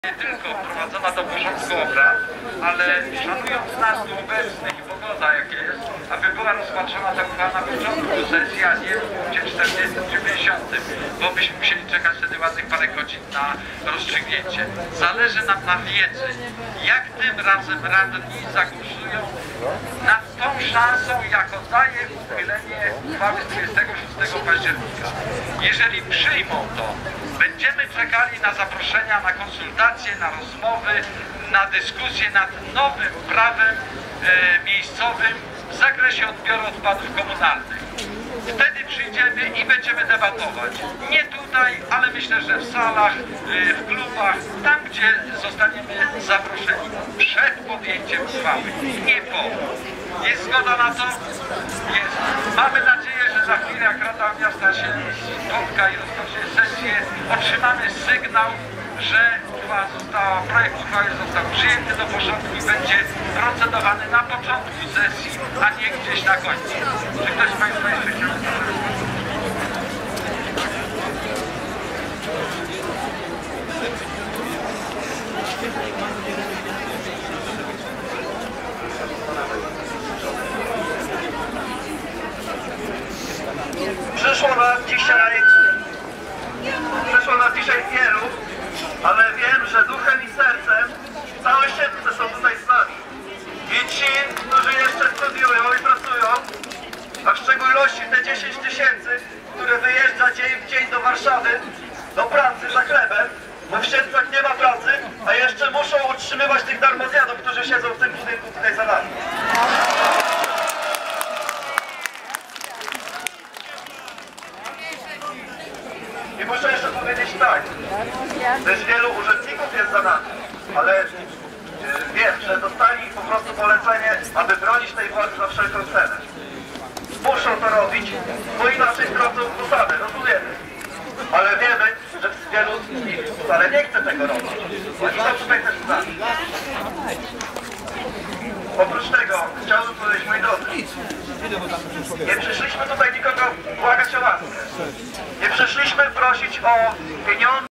Protest w Siedlcach cz.1
Około 300 osób protestuje przed Urzędem Miasta przeciwko uchwale radnych miejskich odnośnie ustawy śmieciowej. Akcja demonstracyjna trwa od godziny 9.30. Nastroje mieszkańców Siedlec odzwierciedlają ostre transparenty typu: „Władza do utylizacji".